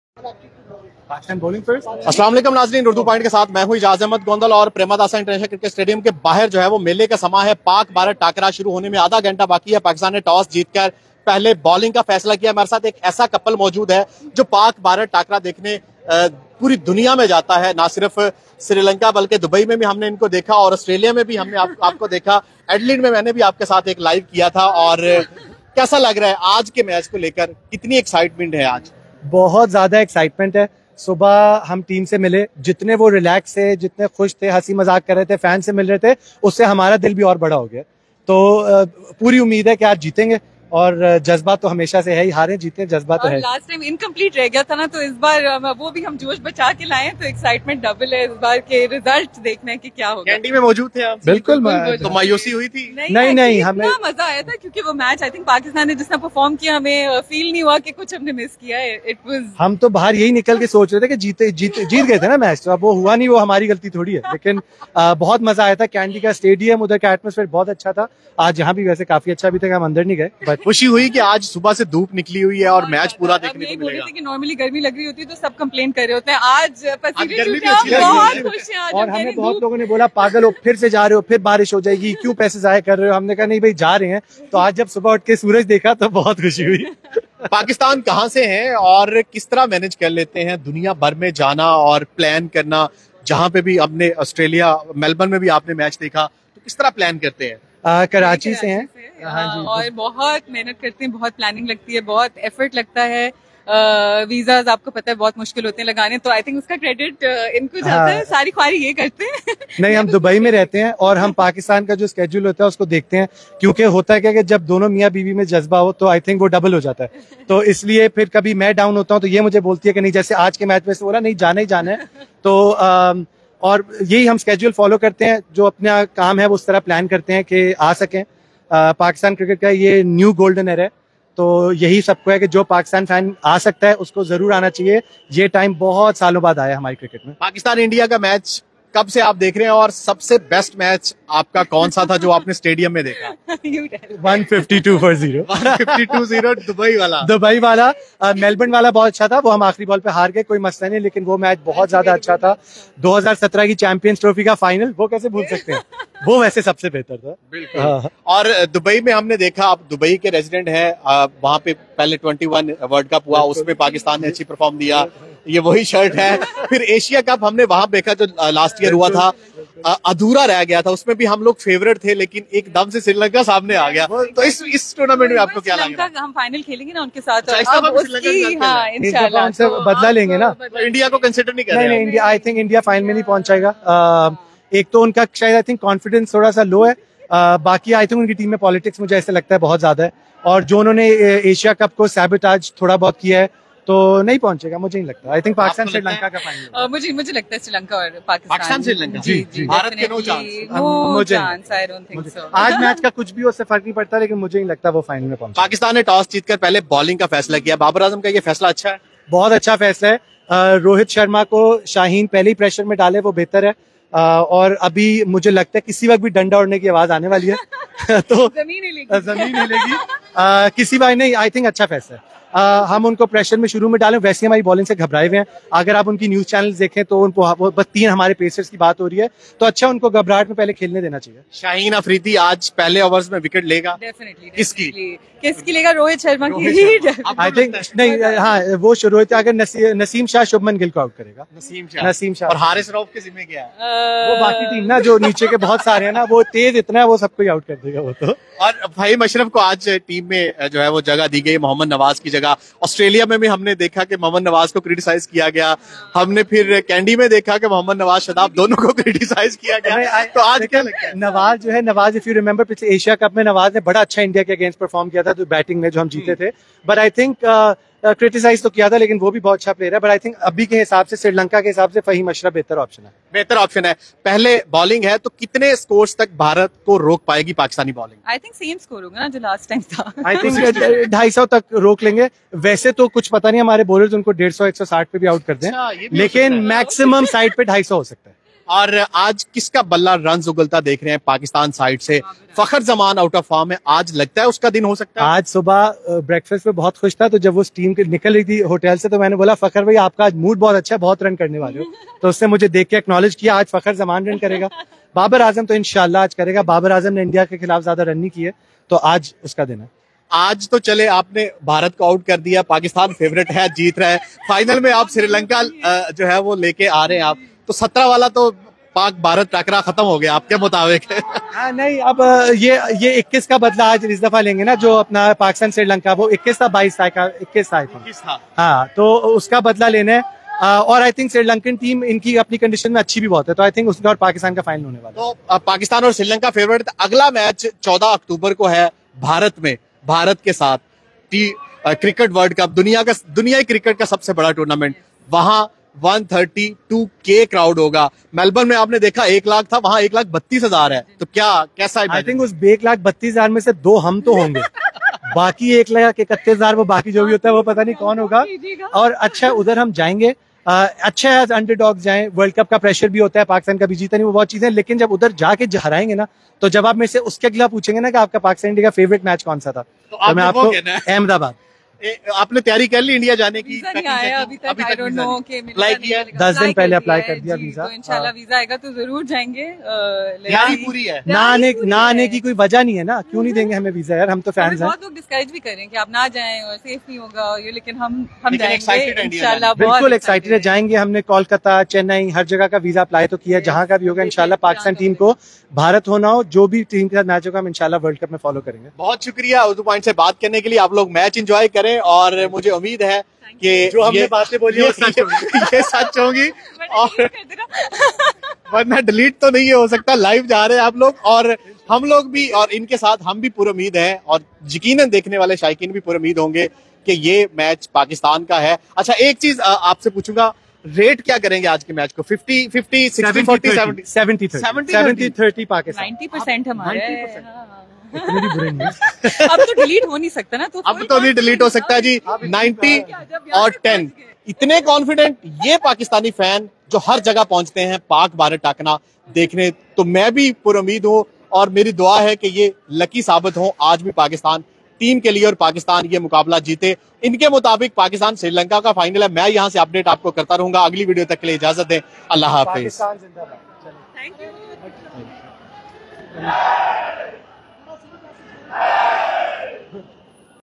Pakistan India Ka Har Match Stadium Mein Dekhne Wala Pakistani Couple
Pakistan India Ka Har Match Stadium Mein Dekhne Wala Mufarid Pakistani Couple

Anchor: Ijaz Ahmed Gondal